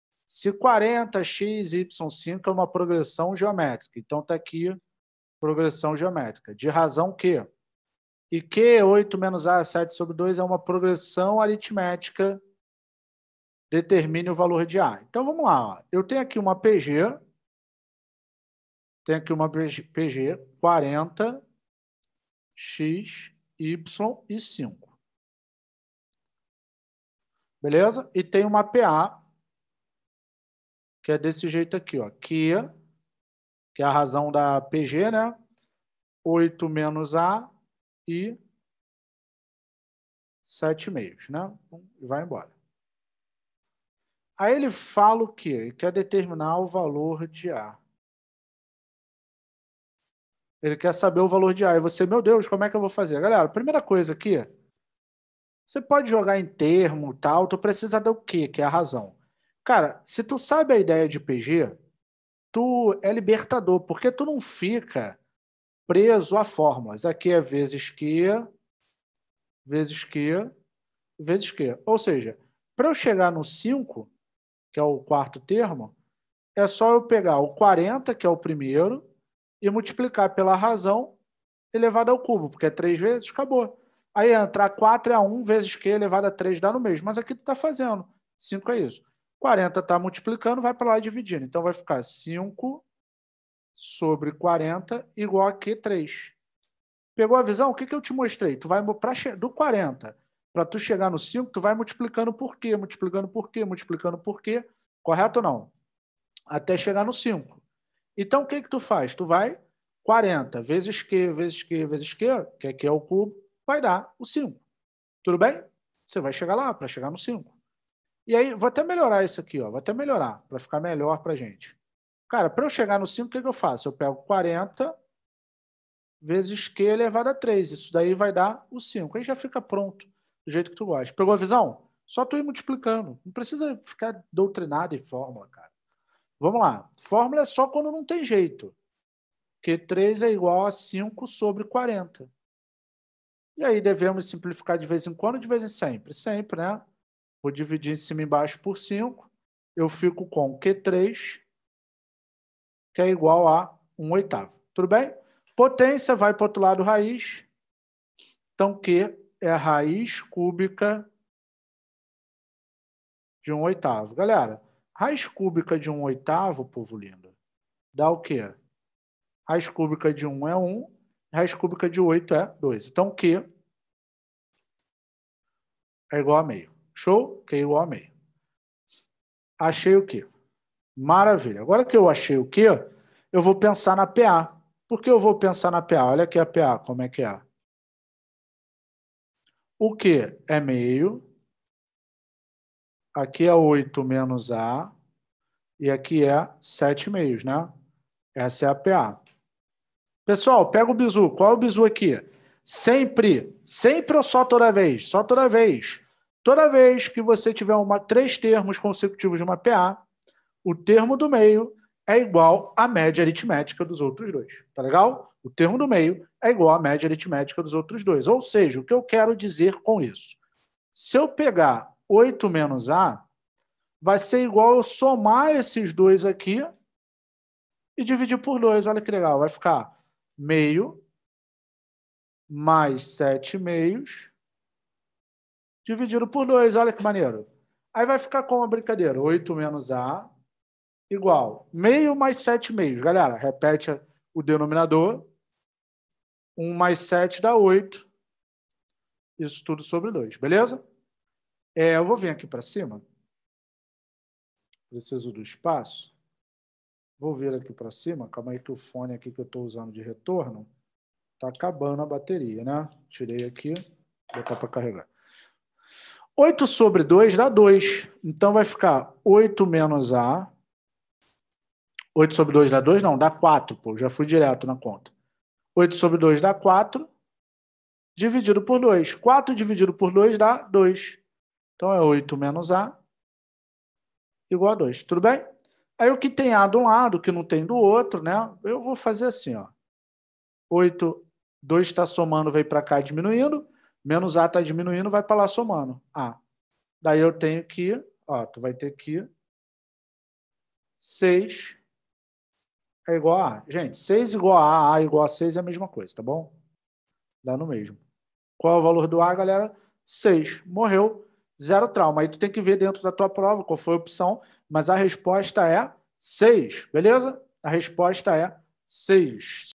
Caiu na Prova da ESA 2020/2021 - Progressão Geométrica.
Se (40, x, y, 5 ...) é uma progressão geométrica de razão q.